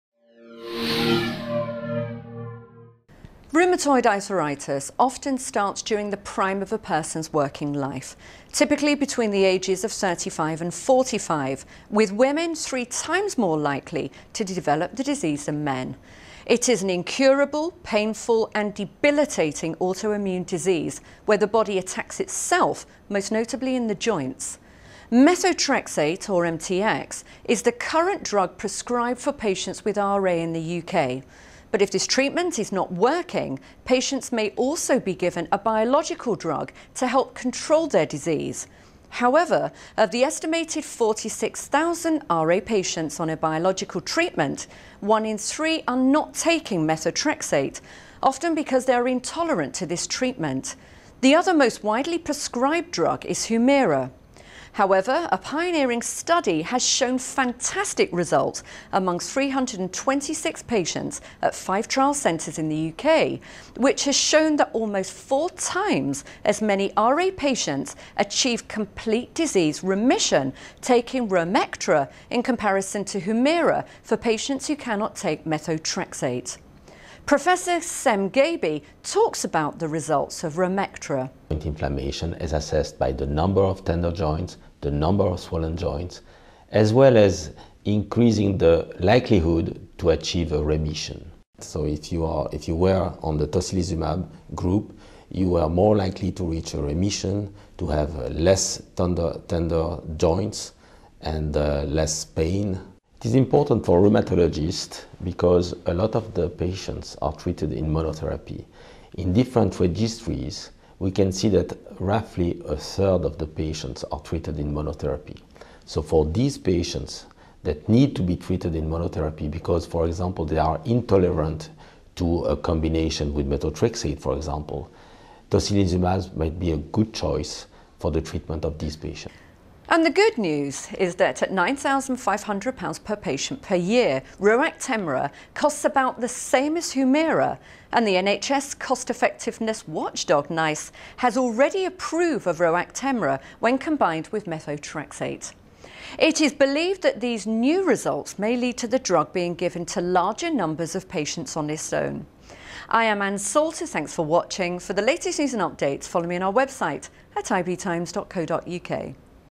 New Drug Gives Hope to Rheumatoid Arthritis Patients
Rheumatoid Arthritis often starts during the prime of a person's working life, typically between the age of 35 and 45.  Women are 3 times more likely to develop the disease than men. It is an incurable, painful and debilitating auto-immune disease where the body attacks itself, most noticeably in the joints.
Methotrexate (MTX) is the current drug prescribed for patients with RA in the UK but if this treatment is not working, patients may also be given a biological drug to help control their disease.  However, of the estimated 46,000 RA patients on a biological treatment, 1 in 3 are not taking methotrexate, often because they are intolerant to this treatment. The other most widely prescribed drug is Humira.
However a pioneering study has shown fantastic results, among 326 patients, at five trial centres in the UK, which has shown that almost four times as many RA patients achieve complete disease remission taking RoActemra in comparison to Humira for patients who cannot take Methotrexate.
Professor Cem Gabey talks about the results of RoActemra.
The good news is that at £9,500 per patient per year, RoActemra costs about the same as Humira and The NHS cost-effectiveness watchdog Nice has already approved of RoActemra when combined with methotrexate.
It is believed that these new results may lead to the drug being given to larger numbers of patients on its own.

Written and Presented by Ann Salter